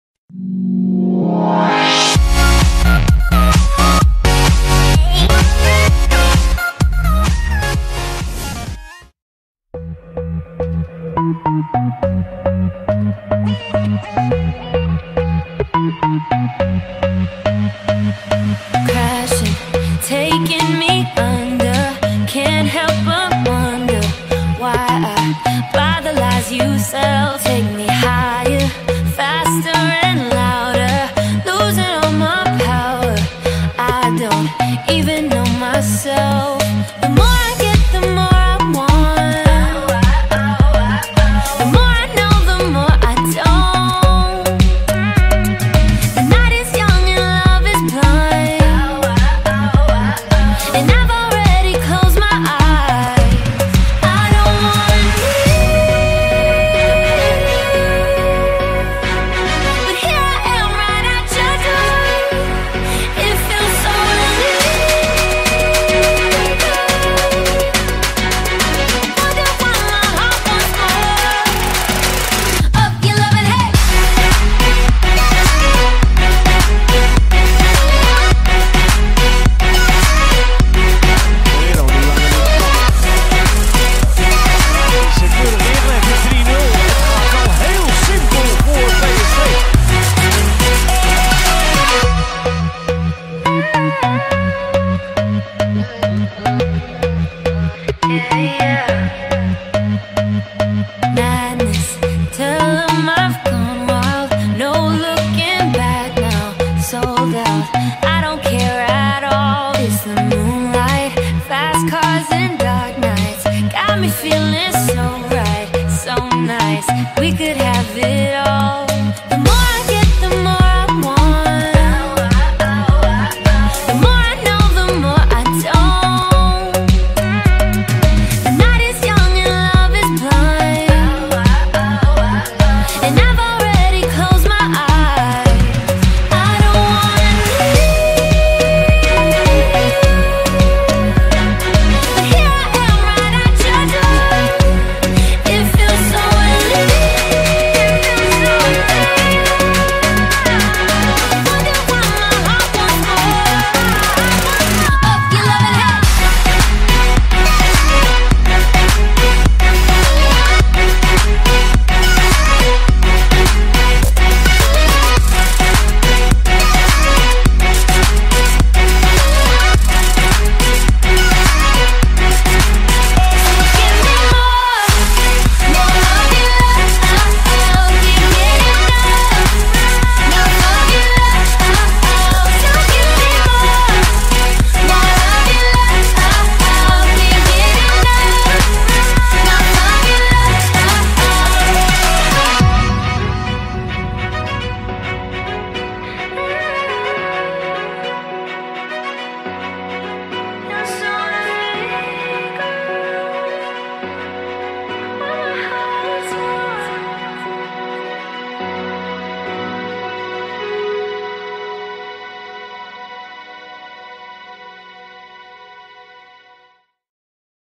Joshua Brenet ►Flying Defender ● PSV Eindhoven ● 16/17 ● ᴴᴰ
Joshua Brenet the right-back of PSV Eindhoven developed rapidly and is working on a good season so far. Recently he made his debut for the Dutch national football team and made much impression. All the highlights of Brenet this season at a glance at PSV Eindhoven and the Dutch national football team.

Personal information
Full name: Joshua Benjamin Brenet
Date of birth: 20 March 1994 (age 22)
Place of birth: Kerkrade, Netherlands
Height: 1.81 m (5 ft 11 1⁄2 in)
Playing position: Right back